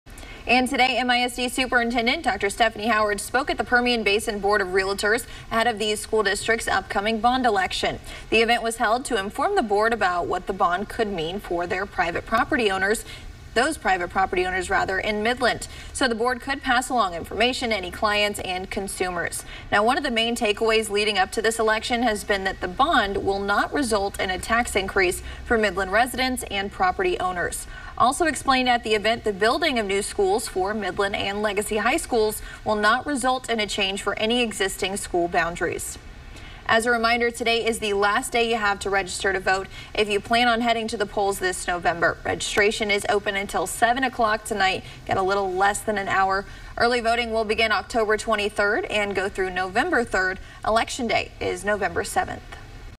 MISD Superintendent speaks on the effects of the bond election
MISD Superintendent Stephanie Howard spoke at the Permian Basin Board of Realtors on the effects the Bond election may have on private property owners.